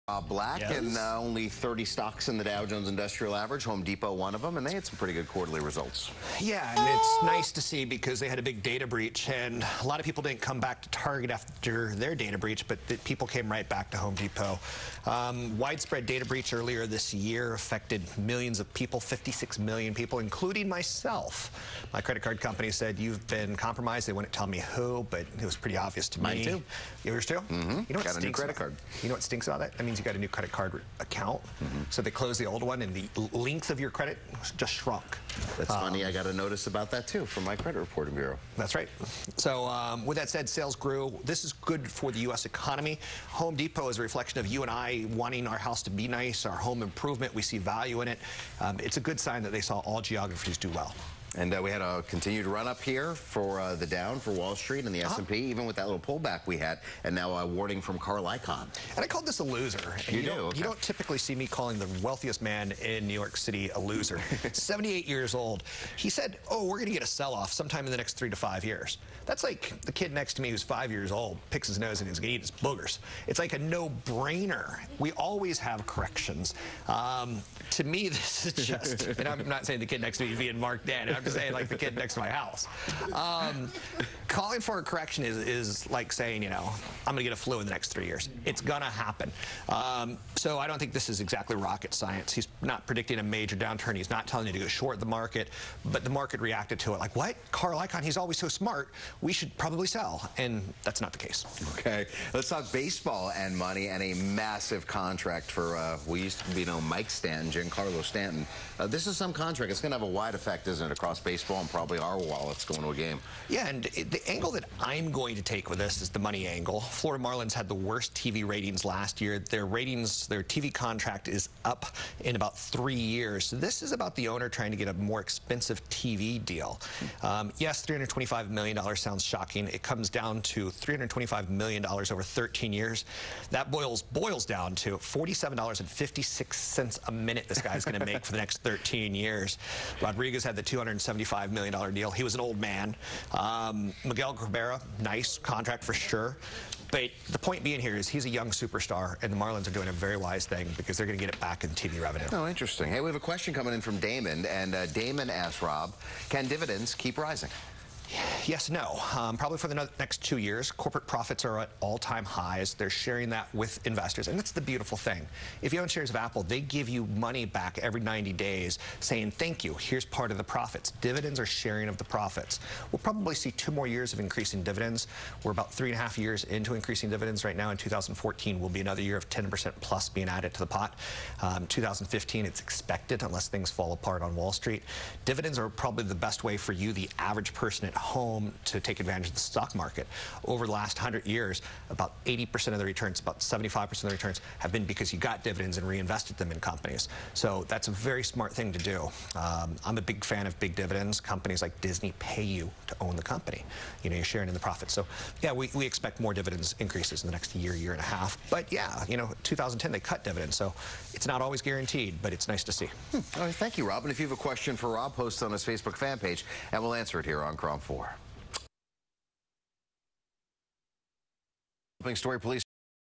Home Depot Results, Icahn'Major correction', Stanton gets $47.56 a minute and dividends
Home Depot's Results, Icahn 'Major correction',  Stanton gets $47.56 a minute for 13 years and Can dividends keep rising?